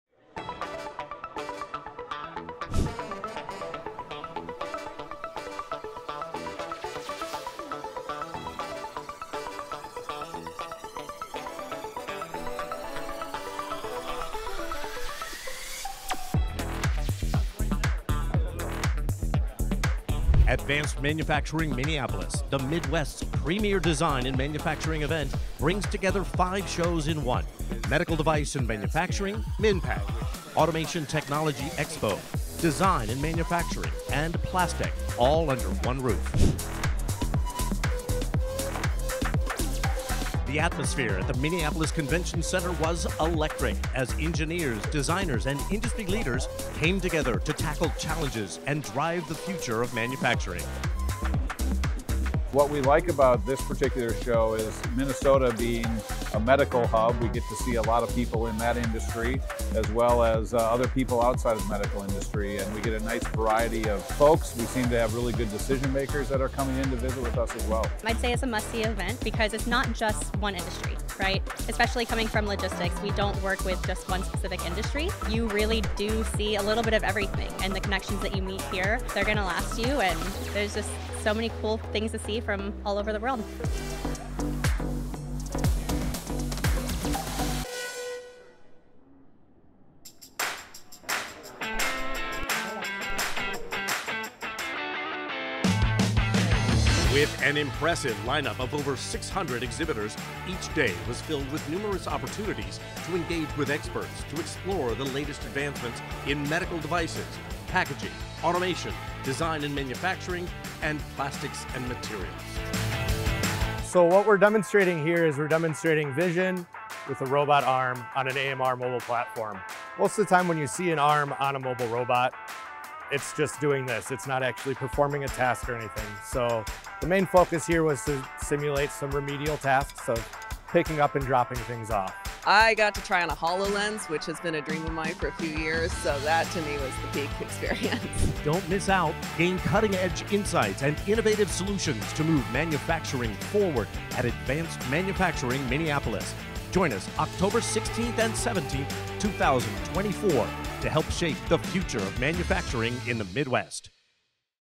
Advanced Manufacturing Minneapolis 2023 Highlight Video
Take a look back at last years Advanced Manufacturing Minneapolis. The Midwest’s Largest Annual End-to-End Design and Manufacturing Event Brought together Medical Design & Manufacturing (MD&M), Automation Technology Expo (ATX), Design & Manufacturing (D&M), MinnPack, and Plastec offering the latest insights and solutions spanning medtech, packaging, automation, plastics, and design.